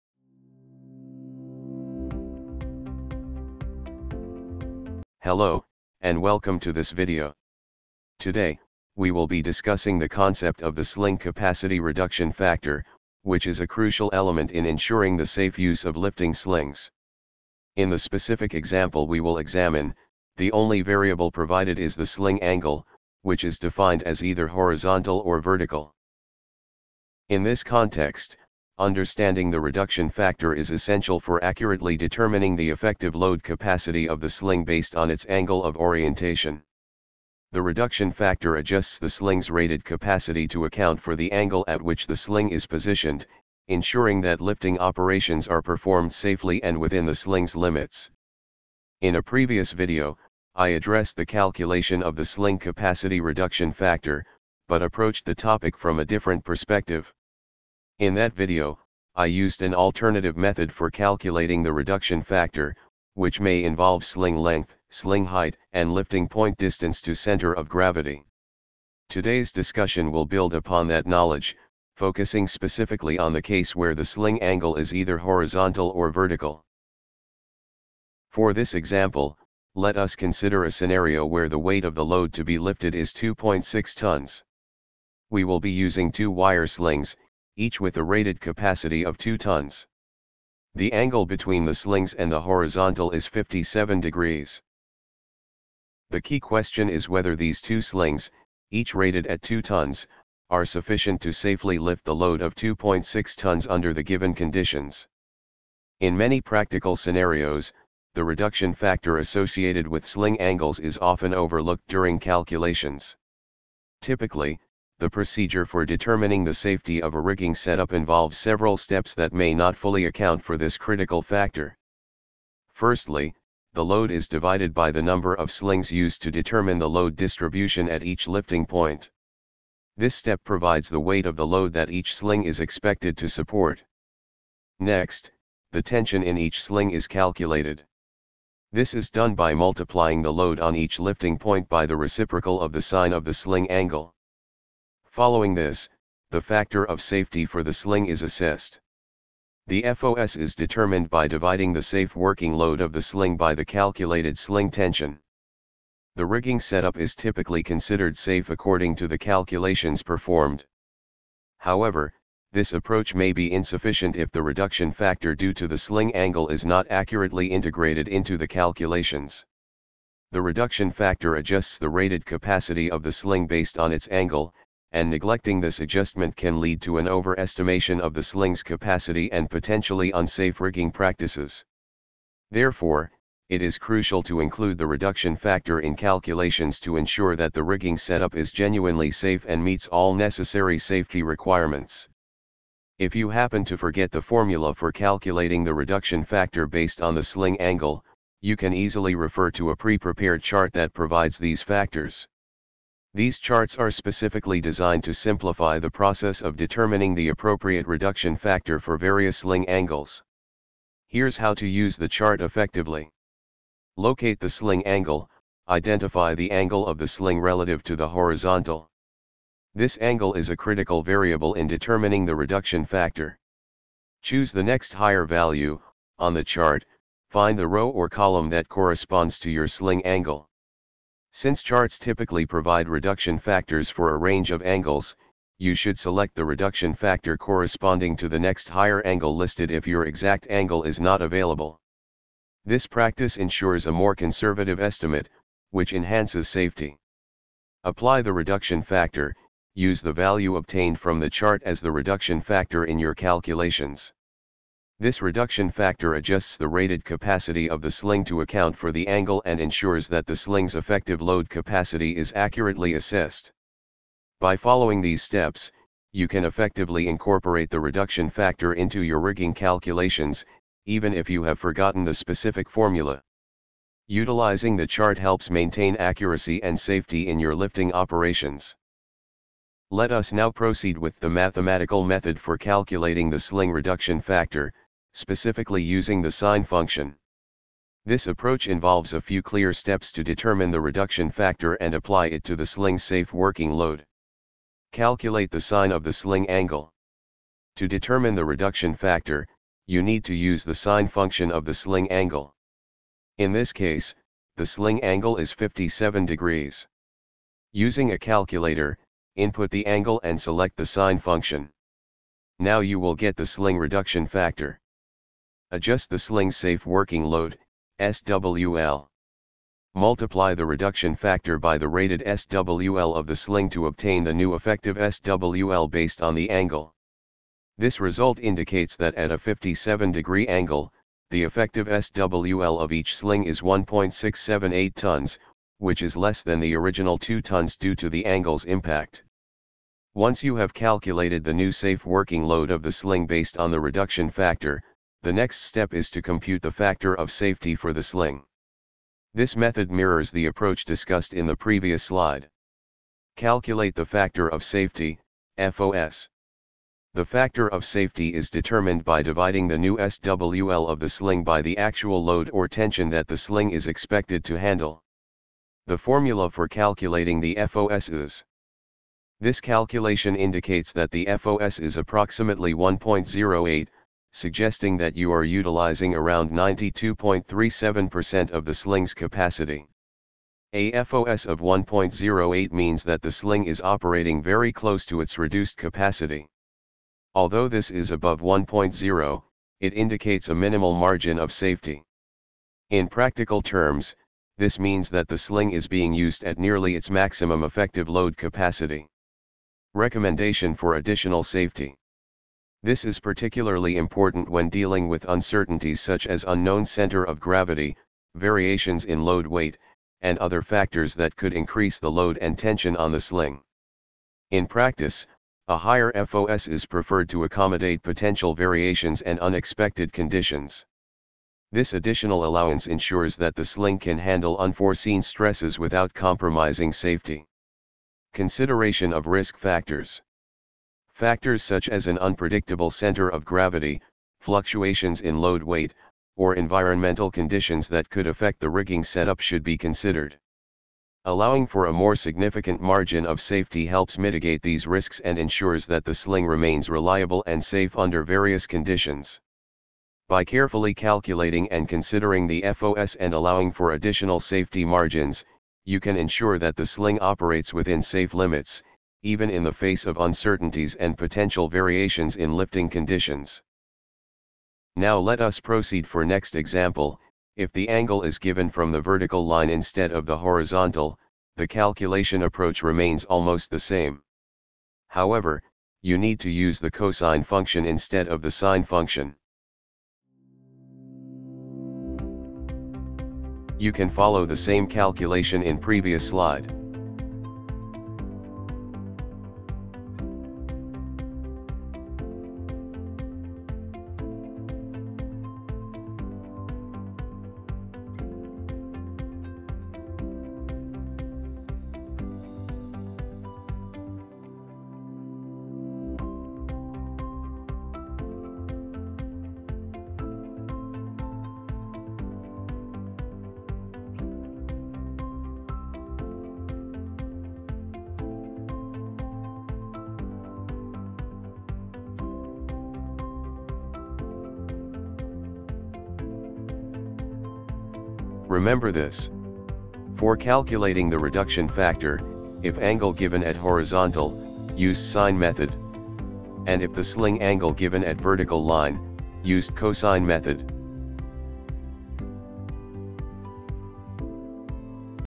Rigging Calculations: How to Determine Sling Reduction Factor
Learn how to calculate the sling reduction factor with this ultimate guide! Whether you're new to rigging or a seasoned pro, this step-by-step tutorial will help you get it right every time.